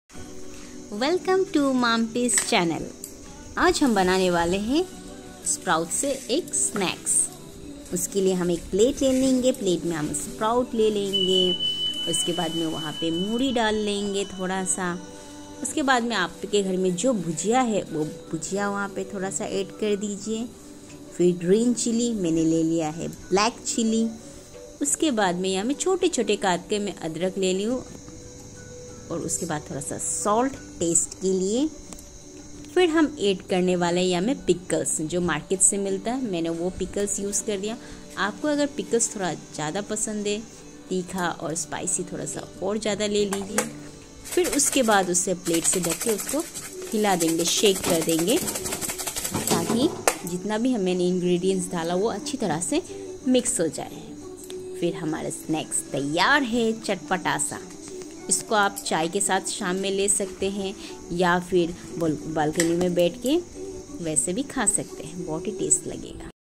Sprout Chaat || Healthy Sprout || Sneak || अंगूरीत मूग 🥬🍽🥄🥣
SPROUT CHAAT RECIPE
(Ingredient)
 Moong(sprout)
Mori
Salt
Pickles
Bhujia
Chilly (chop)
Ginger

shake it well to mix all ingredients then serve in plate